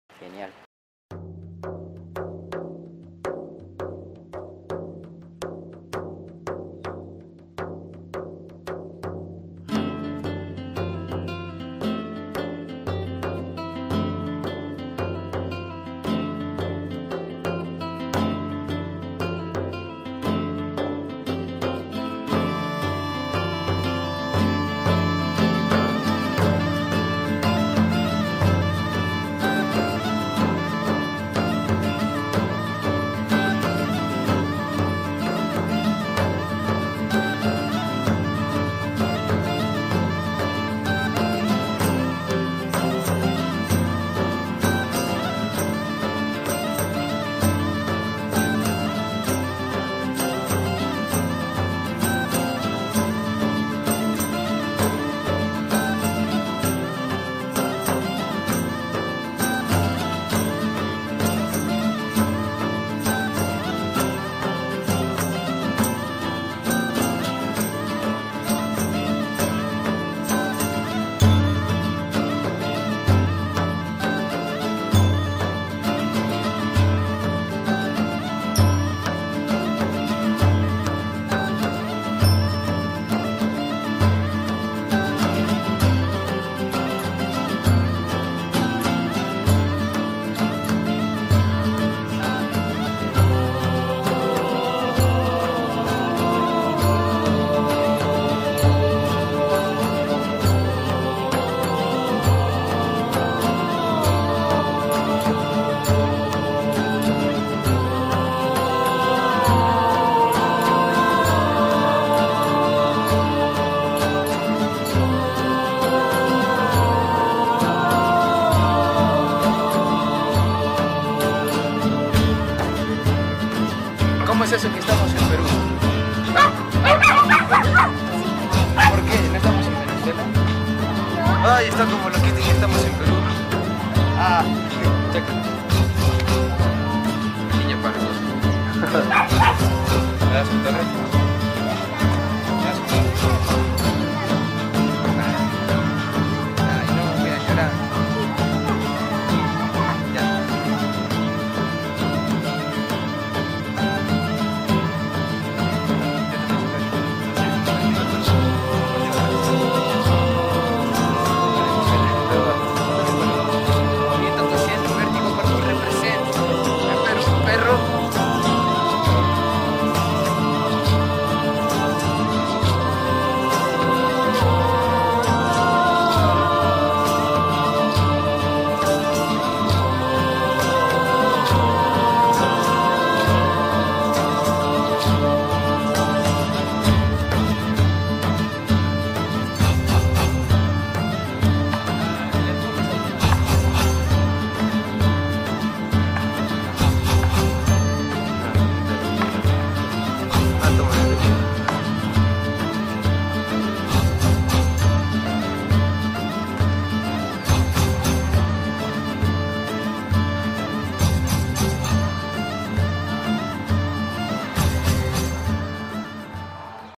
om krisna das en arequipa PARKOUR SALTO EXTREMO
aqui un video maas de nuestra experiencia con el gran om de venuzuela entrenando a full en los parques de arequipa